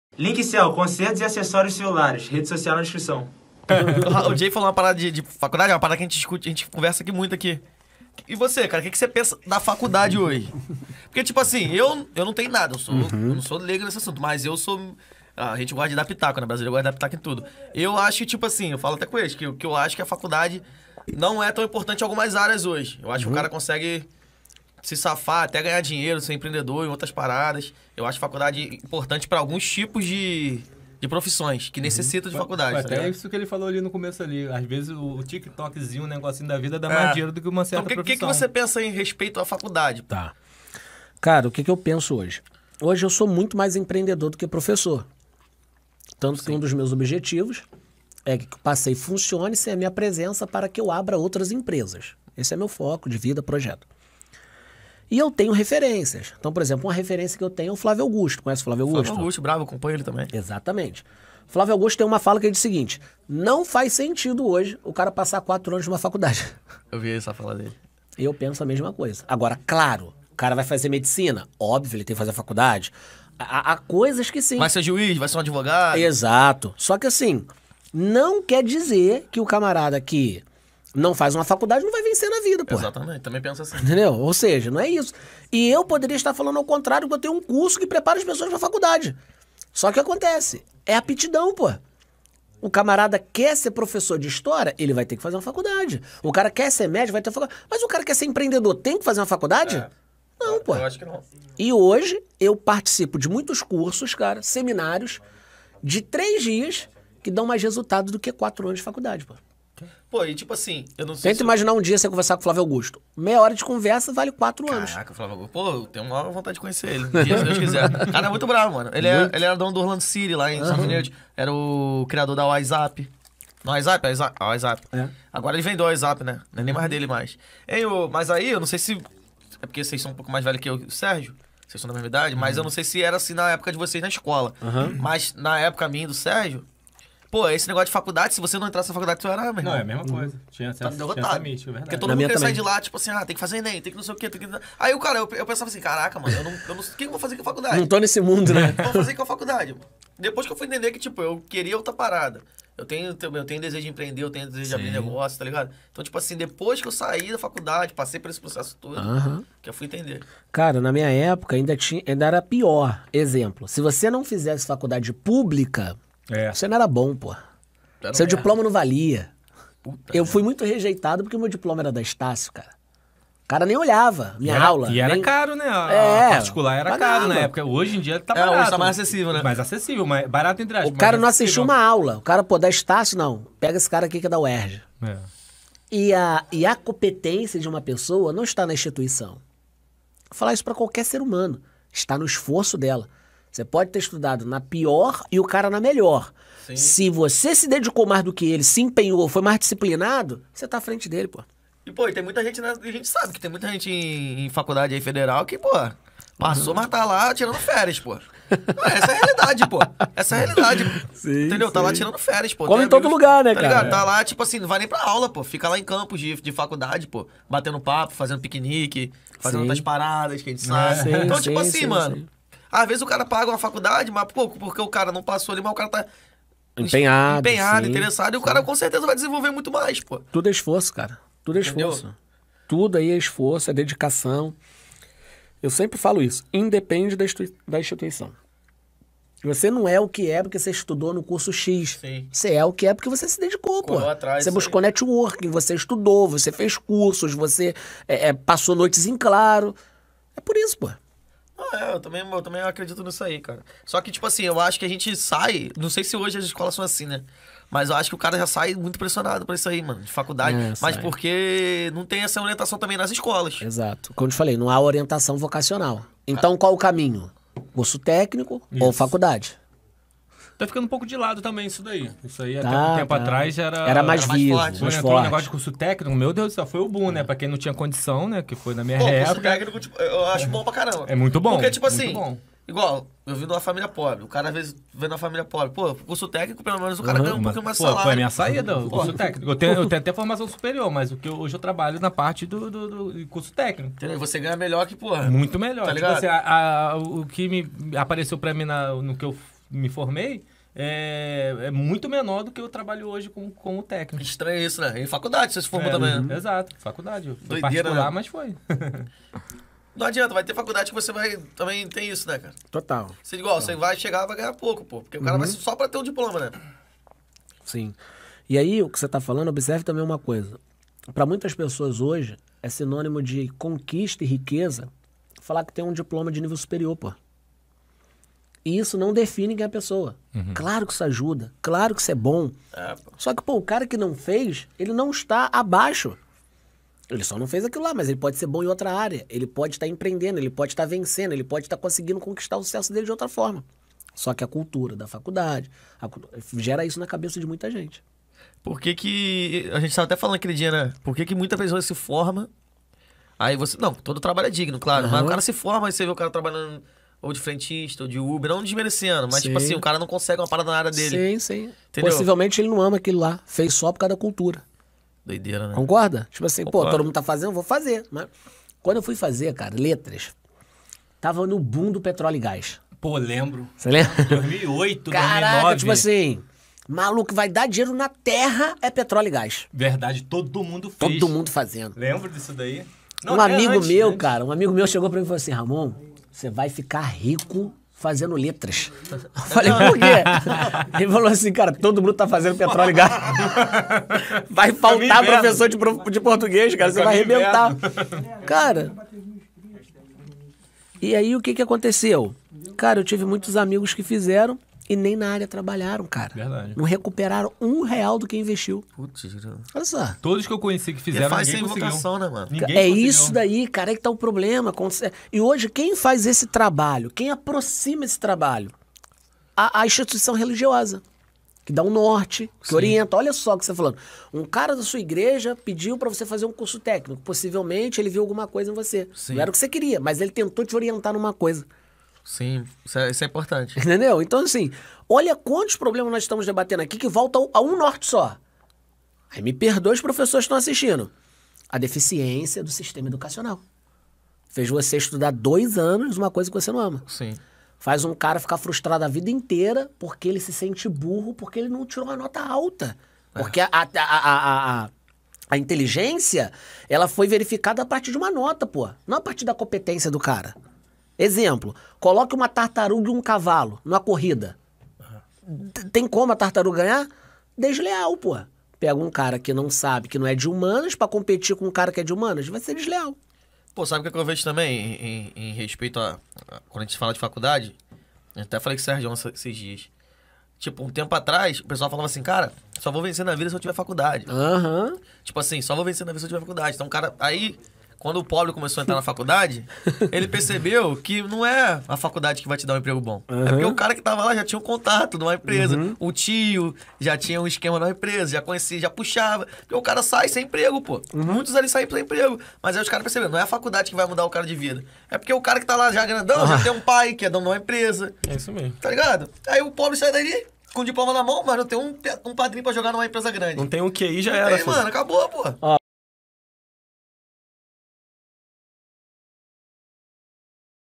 AINDA VALE A PENA FAZER FACULDADE? - Cortes +/- Podcast
Será que ainda é importante a Faculdade? Dá uma olhada.

+/- Podcast - @maisoumenos_podcast

J Silva Material de Construção - @jsilvamateriaisdeconstrucao
Ótica Suzete - @oticasuzete
Link Cell -  @linkcellpbi